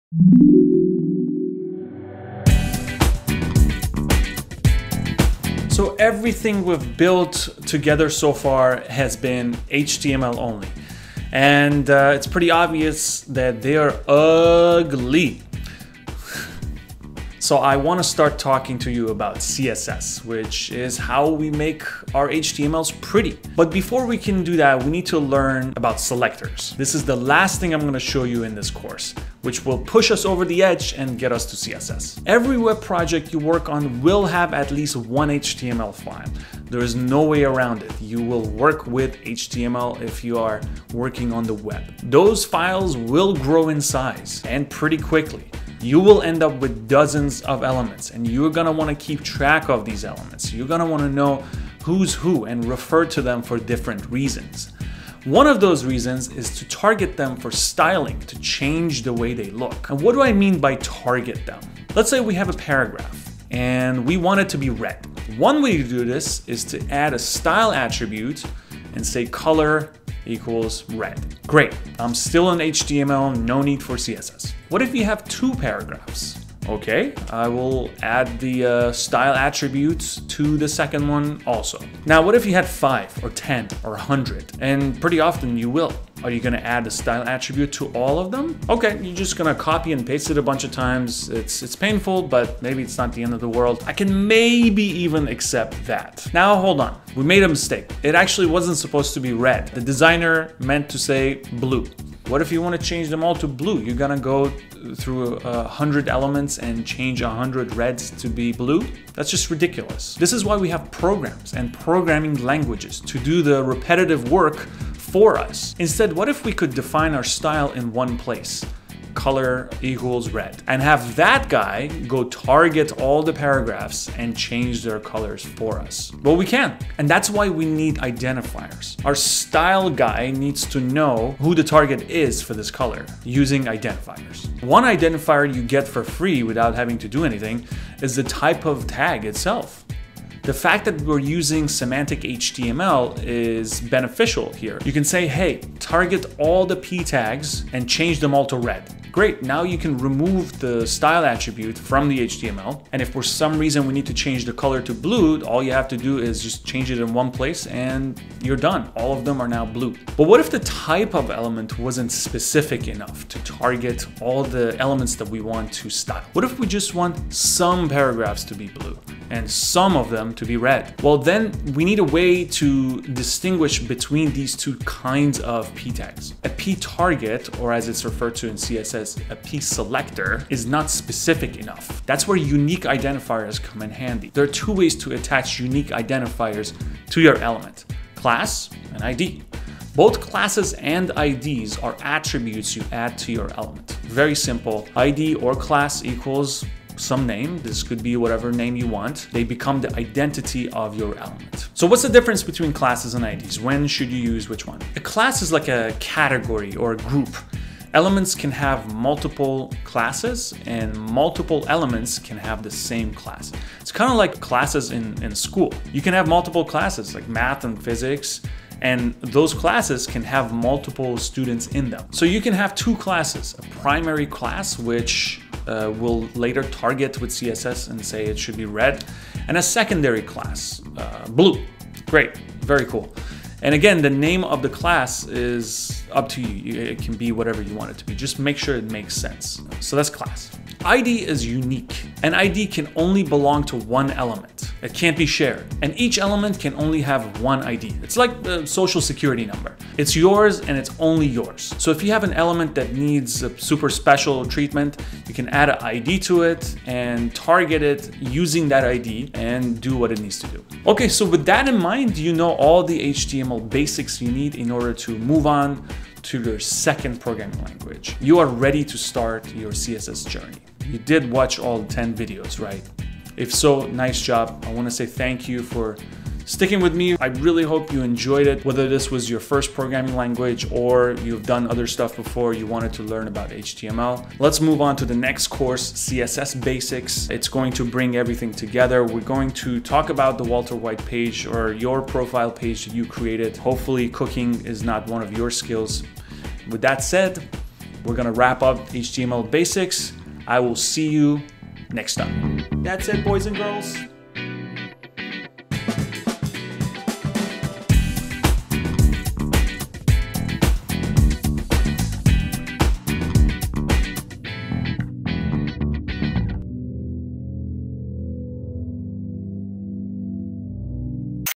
HTML Tutorial — Class, ID, selectors, identifiers (11/11)
So, everything we’ve built together so far has been HTML only. And it’s pretty obvious that they are uuuuuuuu-ugly. So I wanna talk to you about CSS, which is how we make our HTML pretty. But before we can do that we need to learn about selector, … the last thing I’m gonna show you in this course which will push us over the edge and get us to CSS.

Every web project you work on will have at least one HTML file. There is no way around it. You will be working with HTML.

Those files will grow in size pretty quickly. You will end up with dozens of elements and you’re gonna wanna keep track of those elements, you wanna know who’s who and refer to them for different reasons.

One of those reasons is to target them for styling, to change how they look.

What do I mean by target? Let’s say we have a paragraph, and we want it to be red. One way to do is to add style attribute, color: red.

Great. I’m still in HTML. No need for CSS.

What if you have 2 paragraphs? Ok I’ll just add the attribute to both of them I guess.

Now what if you have 5? Or 10? Or 100? And pretty often you will. You’re gonna add style attributes to all of them?

Ok you’re just copy and paste it a bunch of times. It’s painful but maybe not the worst thing in the world. I can maaaaaaaybe even accept that.

Now, hold on. We made a mistake. It wasn’t supposed to be red. The designer meant blue. What if you wanna change them all to blue? You’re gonna go to 100 places in your code and change 100 reds to blue? That’s just ridiculous.

This is why we have programs and programming languages to do our repetitive work for us. Instead what if we could define our styles in one place (color of paragraphs = red), and have that guy go to all of our paragraphs and change the color?

We can, and that’s why we need identifiers. Our style guy needs to know who is the target for this color. Using identifiers. One identifier you get for free is the type of tag itself.

The fact that we are using semantic HTML is beneficial here, as well as all the other benefits we talked about.

You can say hey, target all the p tags, and change them all to red. Great. Now we can remove the style attributes from the HTML, and if for some reason we needed to change the color to blue, all we have to do is change it in this one place, and we’re done.

But what if the type of element wasn’t specific enough? What if you just wanted SOME paragraphs to be red? And SOME to be blue? Well then we need to distinguish between them. And that’s where unique identifiers come in handy. A P target (or as it’s refered to in CSS: p selector) what if that’s not specific enough? There are 2 ways to attach unique identities to elements. Class and ID.

Both CLASSes and IDs are attributes you add to your element. Very simple. They become the identity of your element.

ID or class equals some name, this could be whatever you want, you just can’t have spaces in the name. Letters, numbers, underscore, dash, colon and . are all valid characters.

So what’s the difference between class and ID.

A class is a like category or group. Elements can have multiple classes and multiple elements can have the same class. It’s kinda like classes in school. You can have multiple classes, math, physics, and classes can have people in them. You can add multiple classes to an element by putting space between them. Like this.

So you can have 2 classes here, primary class (which we’ll target in CSS and make it RED, and a secondary class (BLUE). Very cool. And again the name of the class is up to you. Just make sure it makes sense.

So that’s class. ID is unique, an ID can only belong to one element. It can’t be shared. And each element can only have 1 ID. It’s like a social security number. It’s yours and only yours. So if you have an element that needs super special treatment you can add an ID to it and then target that ID to do what you need to do, whether it’s styling, or functionality or whatever else.

Ok so with that in mind, you know all the HTML basics you need, to move on to your second programming language. You are ready to start your CSS journey ??You did watched all 10 videos right? If so, nice job. Let’s move on to the next course. CSS Basics, and I’ll see you there